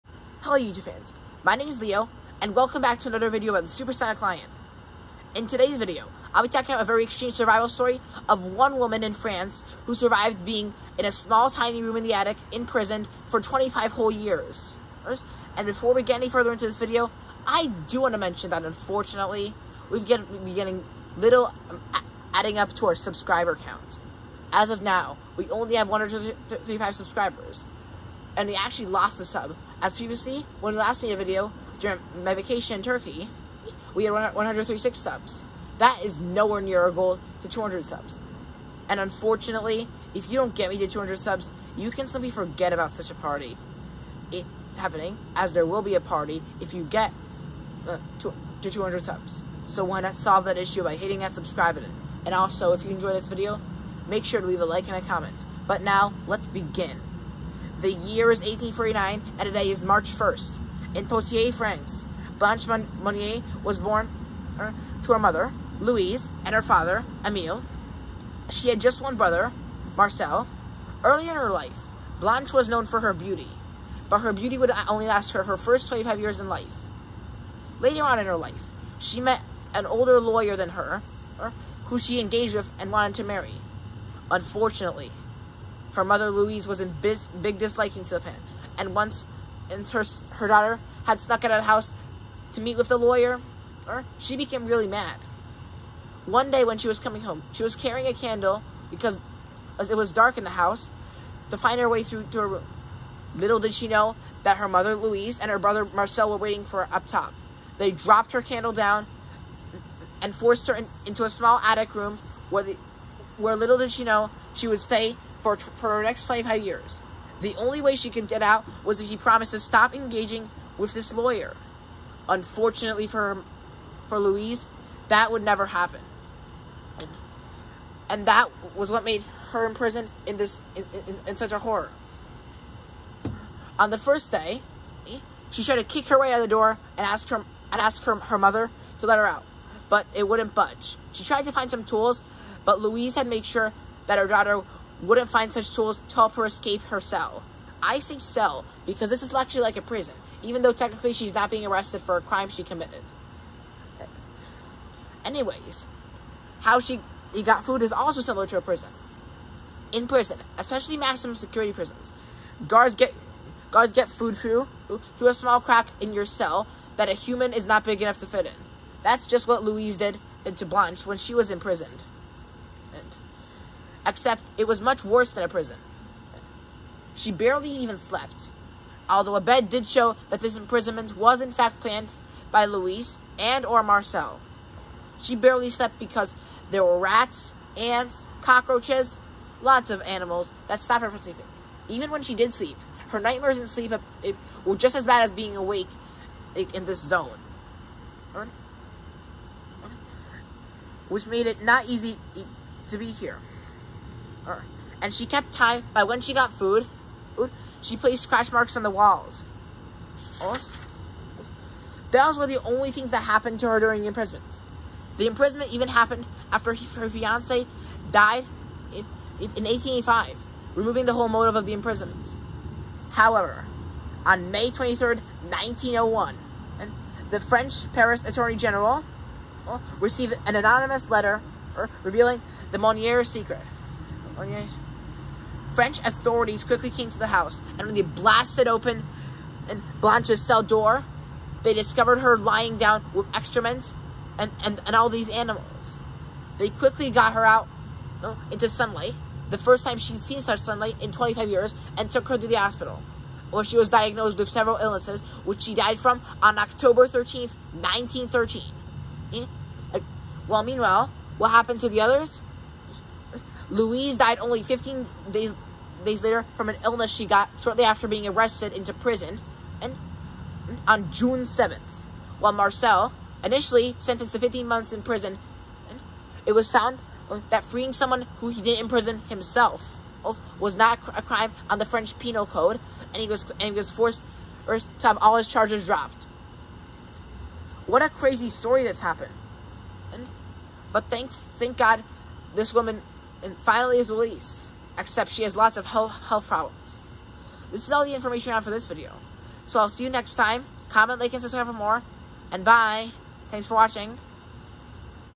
The Insane Way a French Woman Survived 25 Years Locked in a Small Attic Room (9/1/22)
Hello YouTube Fans. My name is Leo and welcome back to another video by the Supersonic Lion. In this video, I will be talking about the insane story of the French woman who was locked in a small attic room for 25 years and survived.